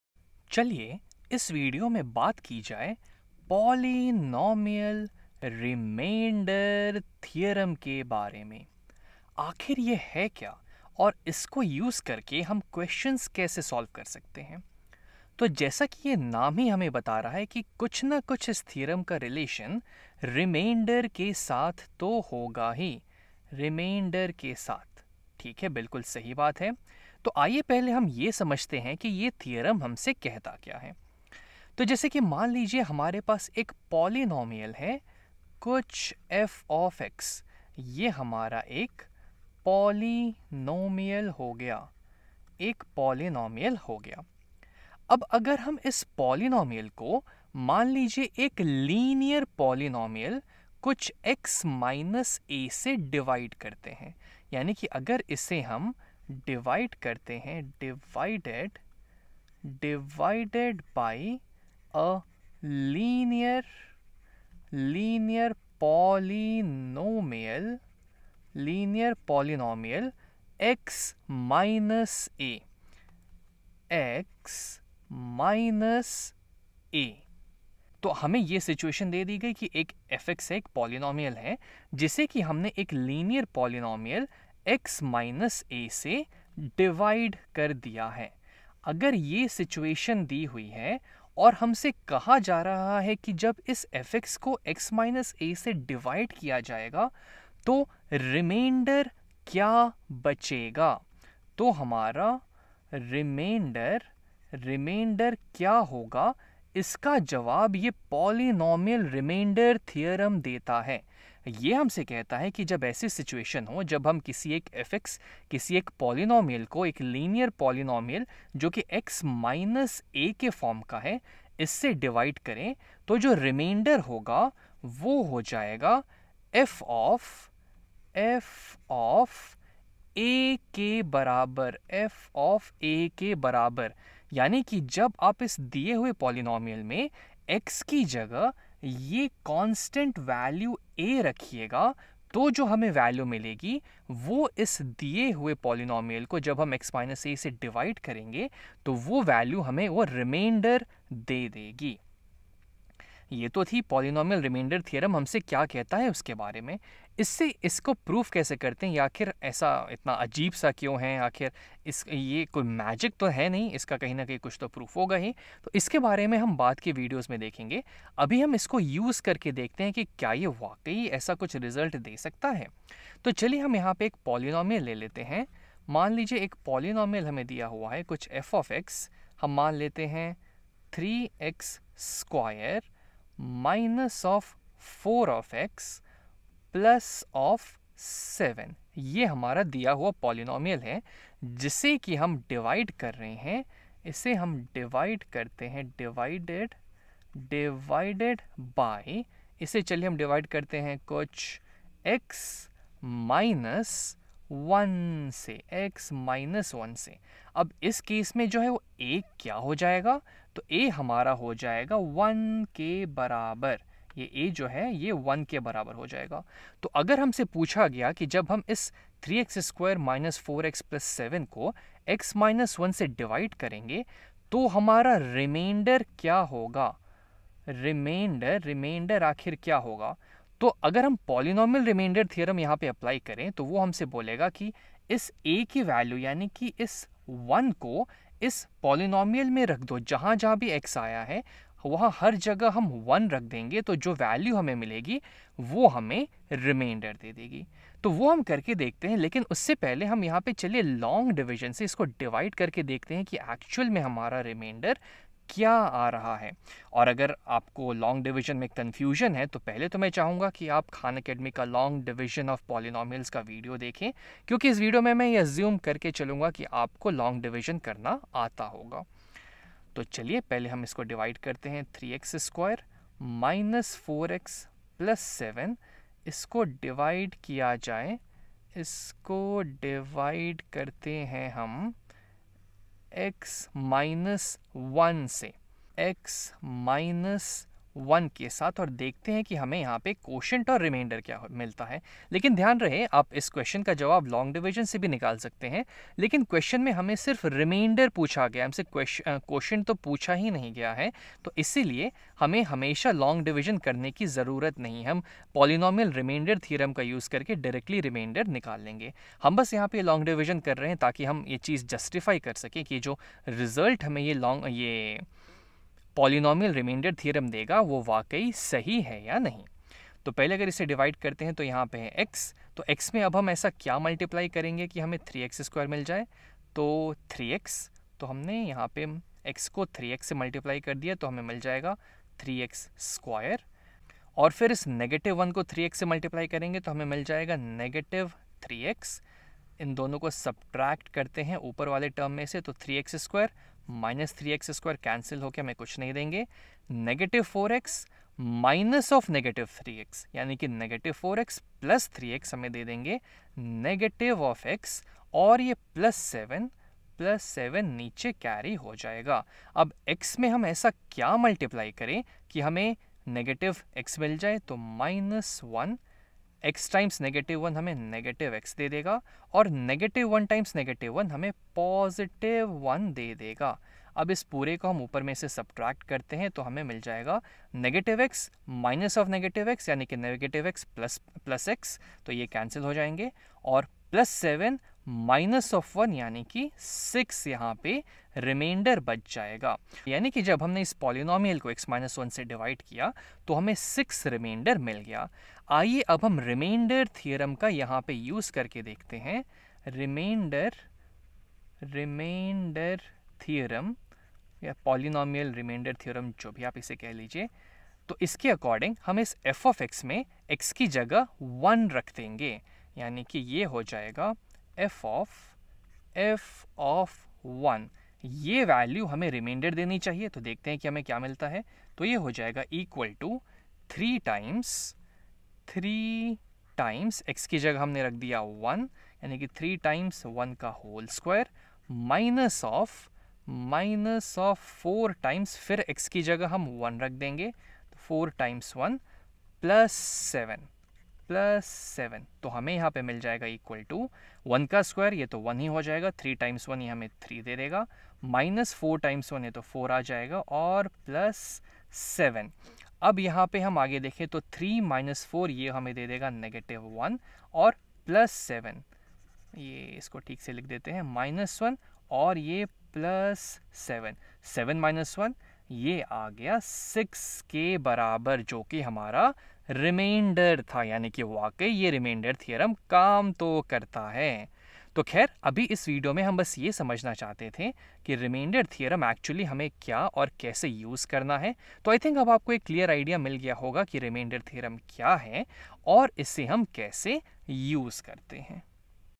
Intro to the Polynomial Remainder Theorem (Hindi)
The Polynomial Remainder Theorem allows us to determine whether a linear expression is a factor of a polynomial expression easily. Check it out!